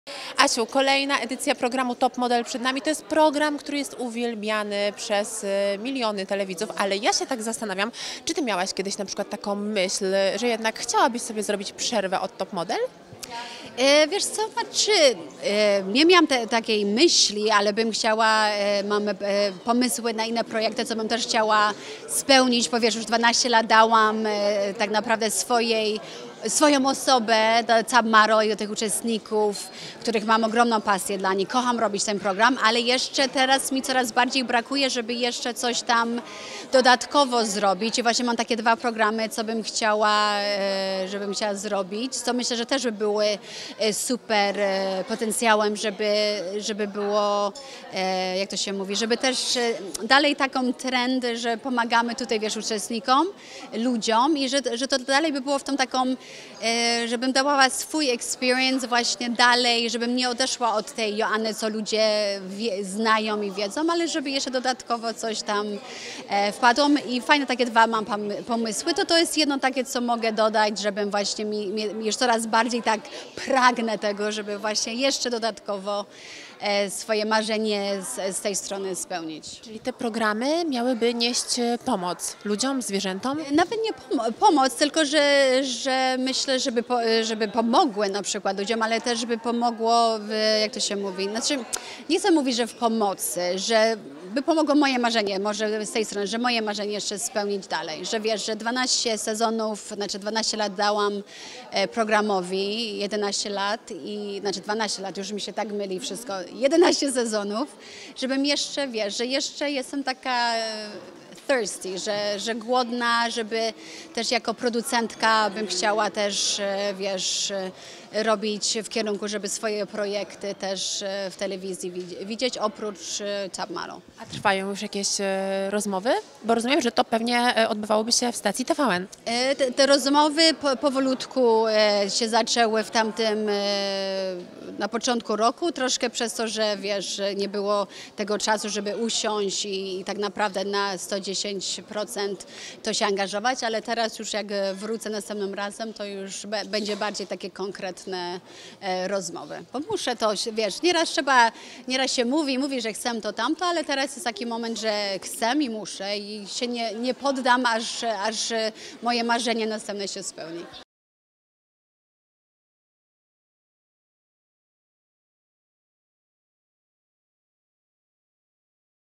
Czy Joanna Krupa jest zmęczona programem "Top Model"? Ma chrapkę na inny program?
Joanna Krupa od 12 lat występuje w programie Top Model w roli współprowadzącej i jurorki. Gwiazda zdradziła nam, czy po tak długim czasie spędzonym na planie show odczuwa zmęczenie. Okazuje się, że chciałaby spełnić się w nowej roli!